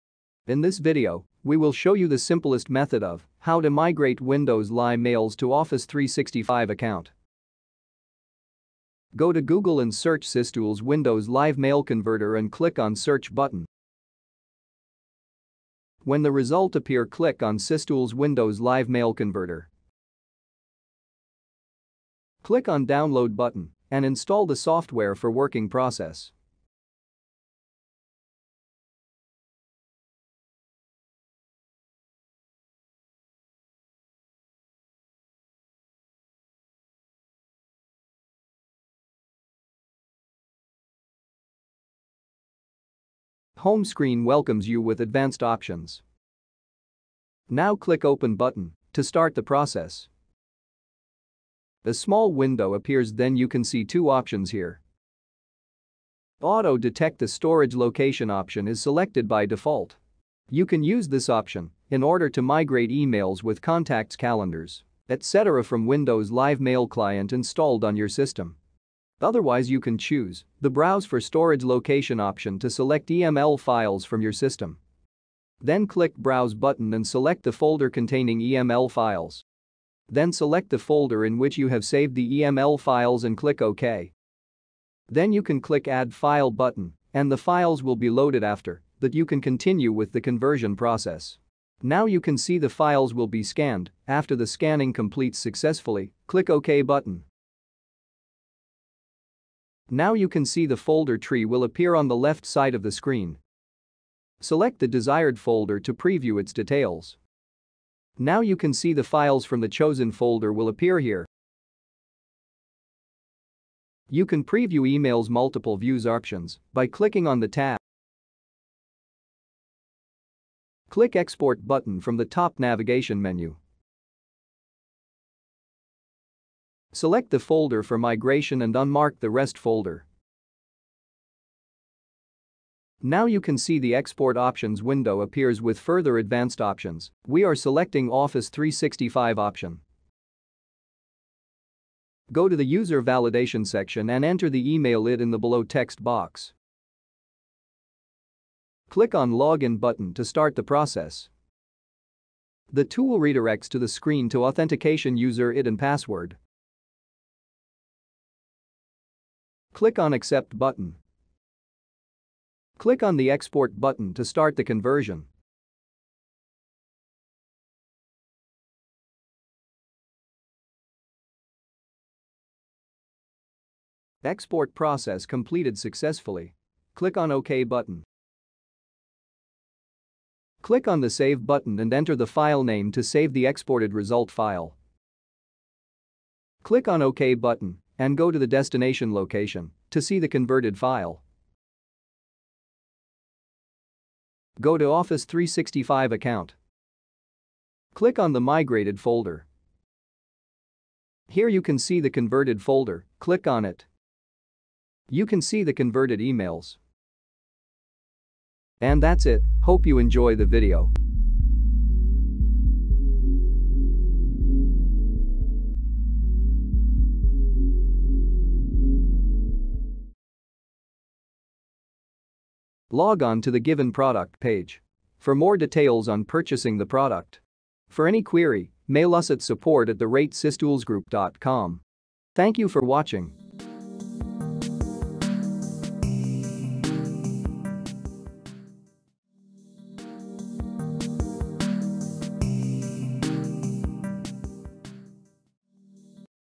Windows Live Mail to Office 365 Migration Tool - Import WLM into Office 365
MS Office 365 represents a combination of online services and apps. Microsoft came up with the idea of using apps and related services to group your business. It has amazing features to use Excel, PowerPoint and Word etc without having to install it on your physical computer.

We find many questions and people are looking for a way to import emails from Windows Live Mail to their Office 365 accounts, so we recommend using Windows Live Mail to Office 365 Migration Tool, which allows you to import WLM emails to Microsoft 365 account.